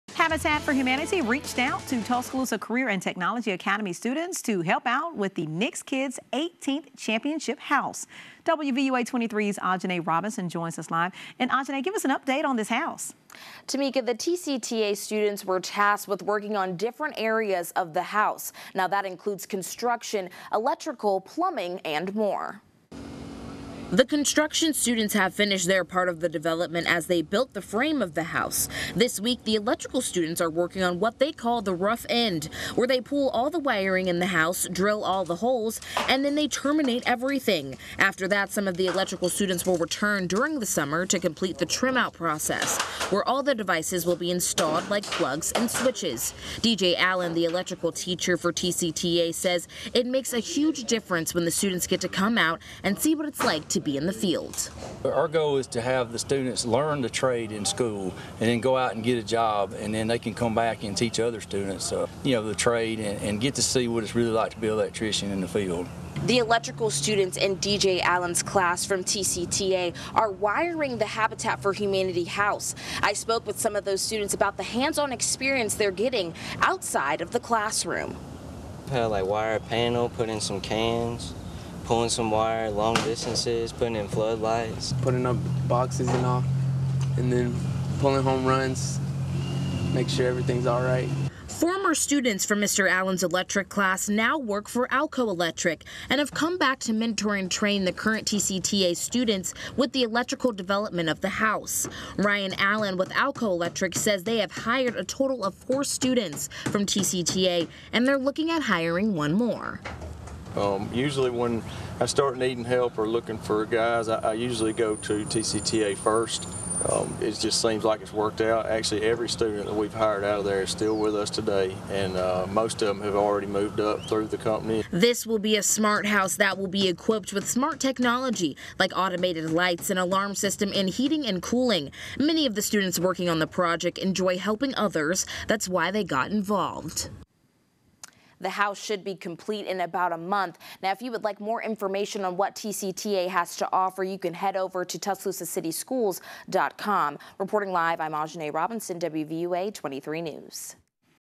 Tuscaloosa Students Help Wire Habitat for Humanity House
Habitat for Humanity of Tuscaloosa needed help constructing their 18th Nick's Kids Foundation championship house. Students at the Tuscaloosa Career and Technology Academy needed experience in their fields. So the organization is teaming up with the school to build houses and future careers.

The TCTA students are being tasked with different areas of the house to work on including construction, electrical and plumbing.